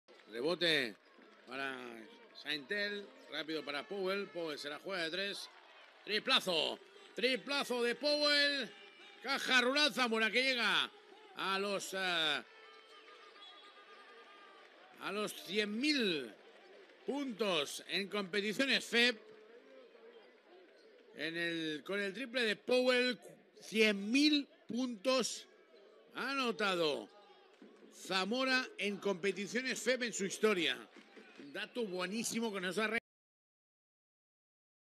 Jahvaughn Powell anota el punto 100000 para el CB Zamora
¡Y llegó! @jp_smooth1 no perdonó y enchufó el punto 1️⃣0️⃣0️⃣.0️⃣0️⃣0️⃣ en la historia del

Hito histórico ¡Y en casa!

📹 Competiciones FEB (X)